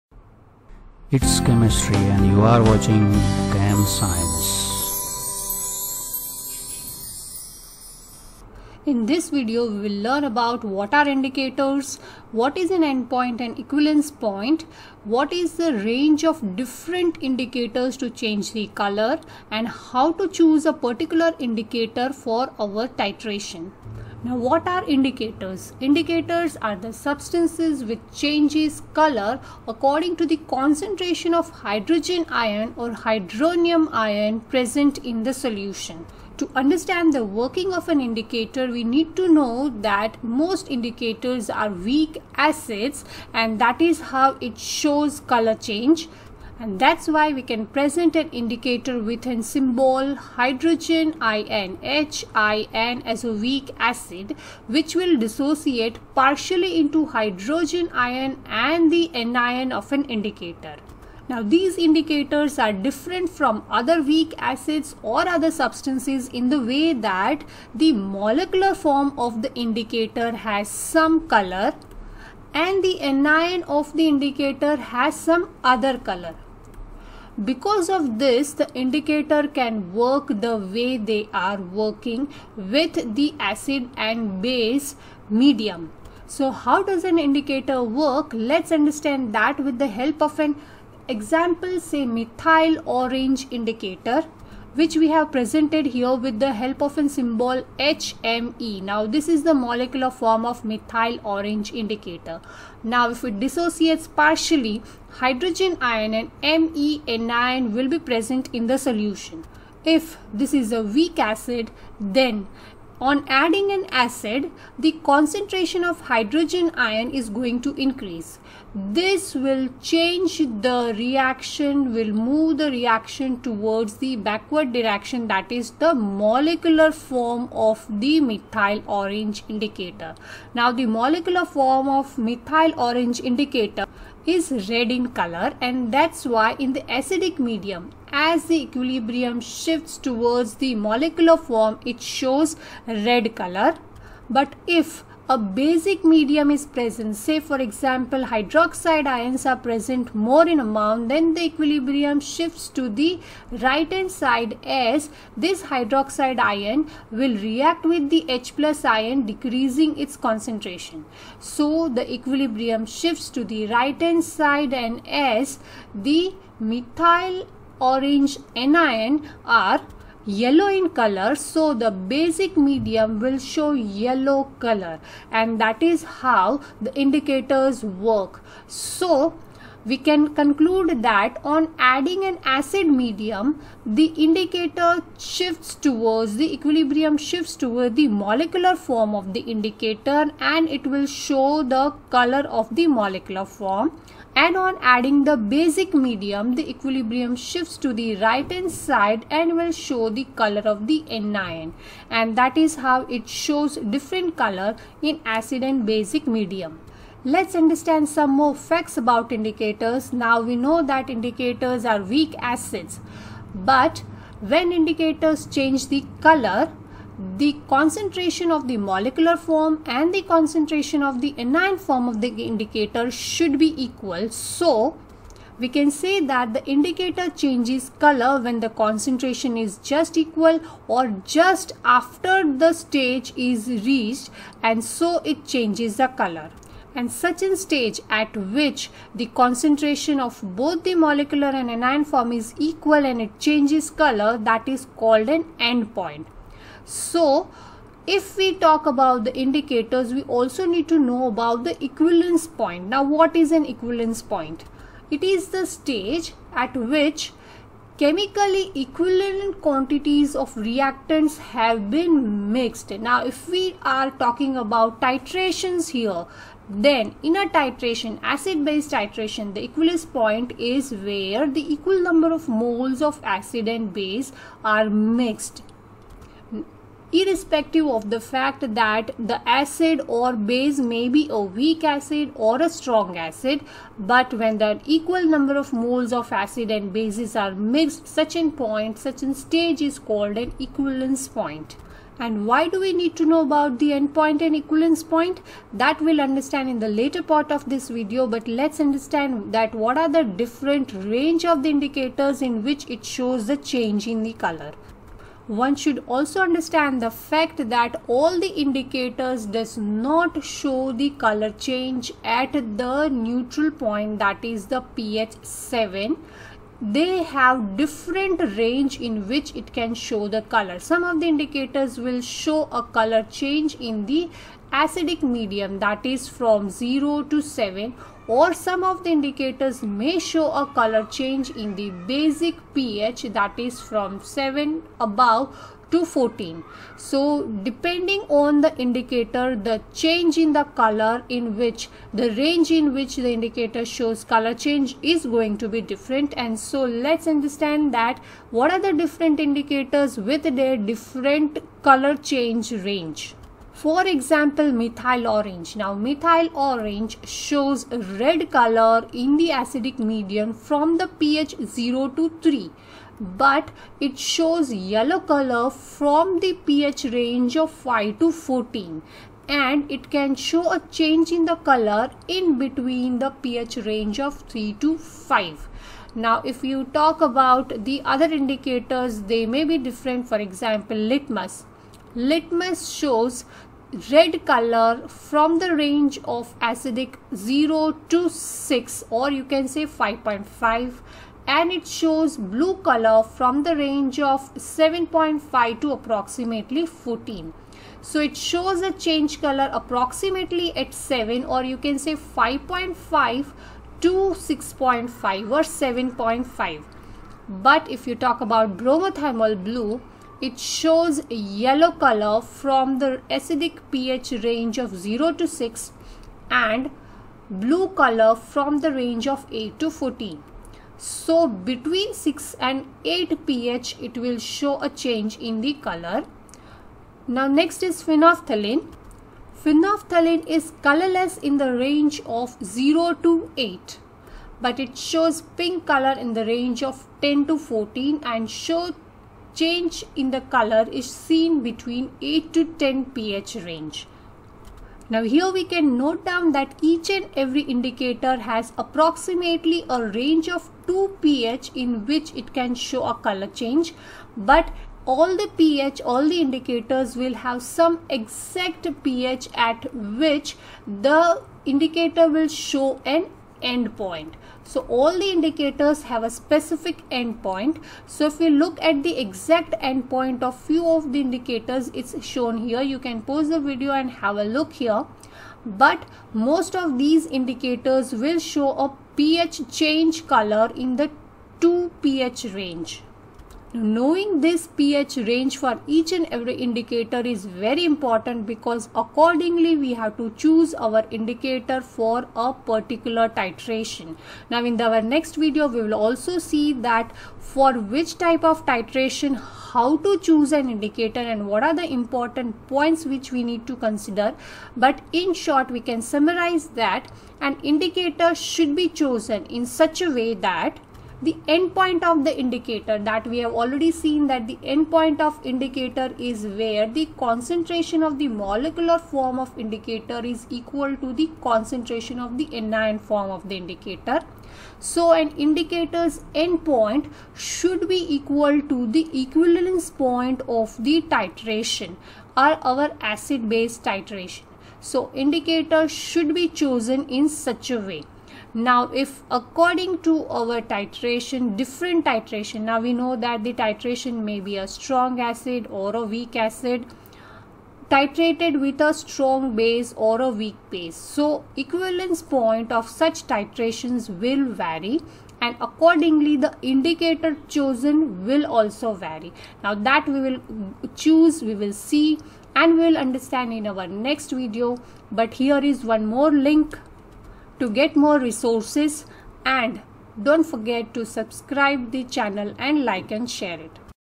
Ionic equilibria 03: Indicators, range of indicators, : A levels/IB/ class 12 chemistry
This video explains what are indicators, what is an end point and equivalence point and the difference. it also explains the range of color change for various indicators and how to chose an indicator.